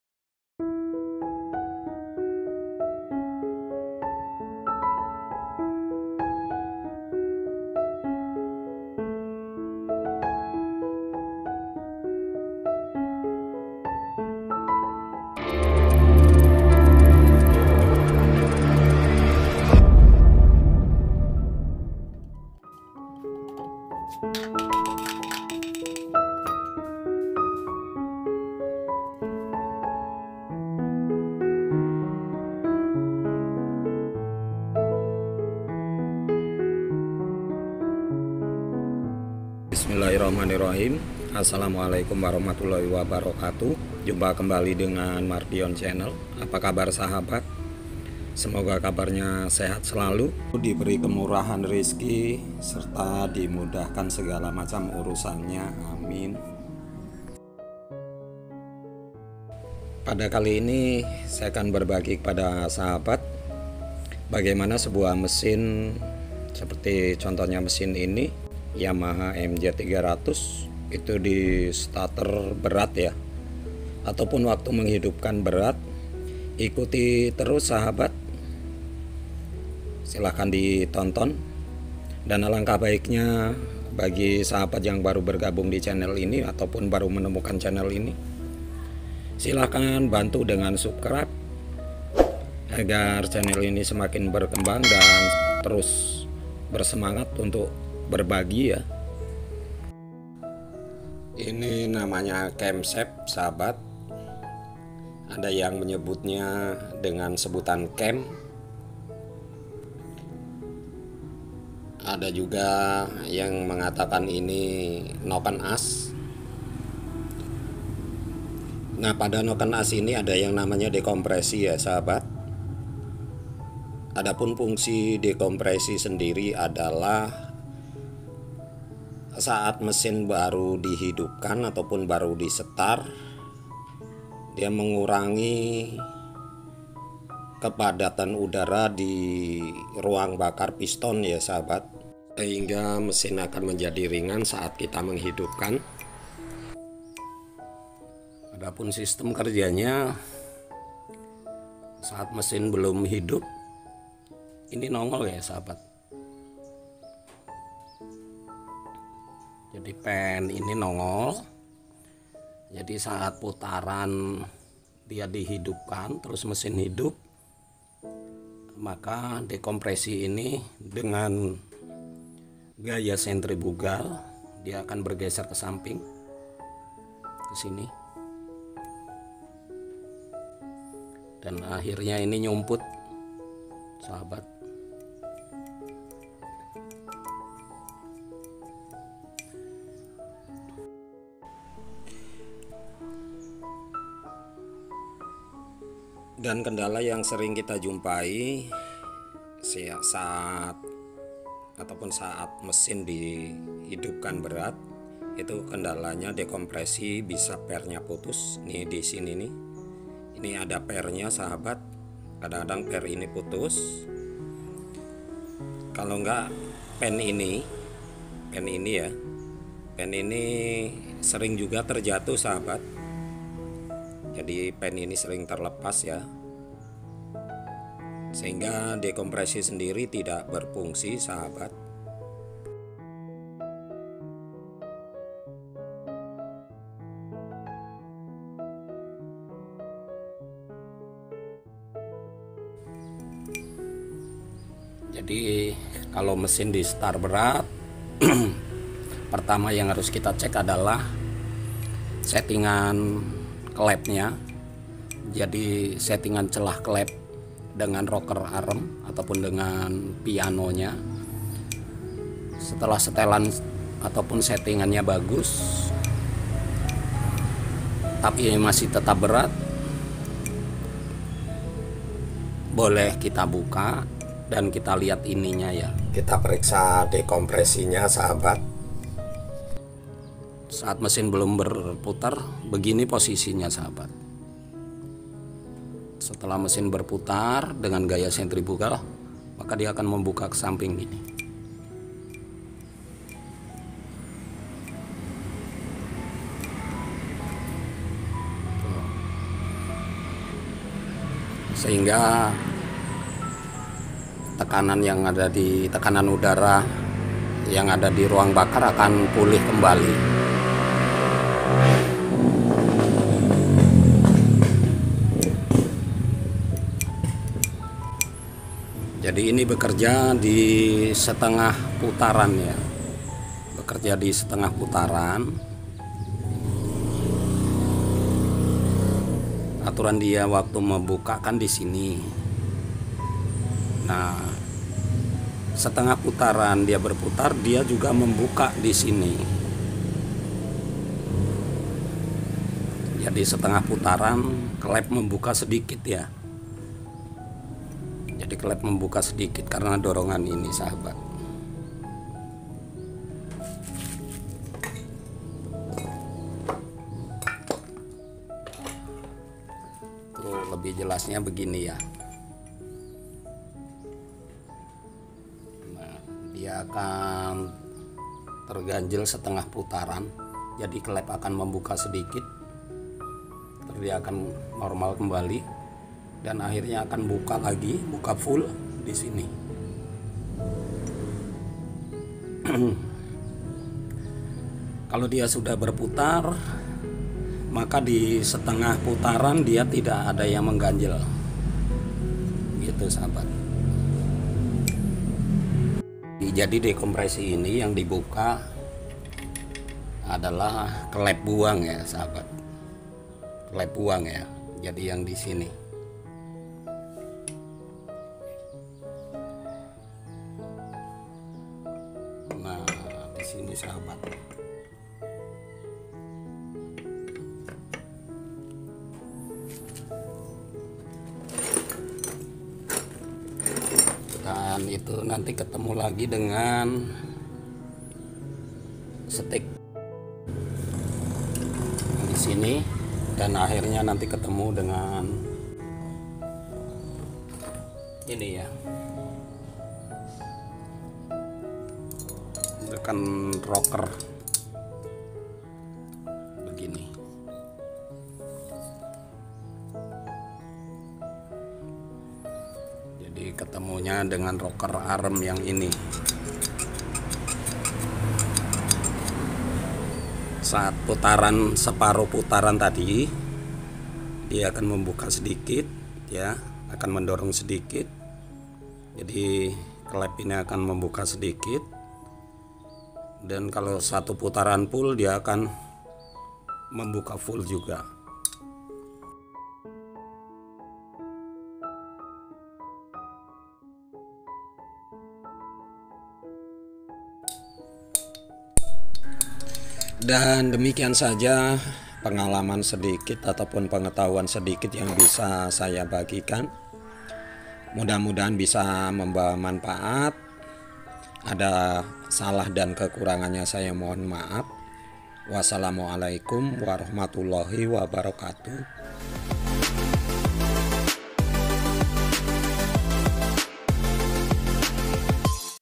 belajar mengenali fungsi dan sistem kerja dekompresi
fungsi dekompresi pada mesin penggerak seperti mesin MZ 300 adalah untuk mengurangi kepadatan udara di ruang bakar pada mesin. sehingga mesin menjadi ringan saat di hidupkan. tapi setelah mesin hidup dekompresi akan membuka dengan gaya sentribugal sehingga kepadatan udara di ruang bakar akan kembali utuh.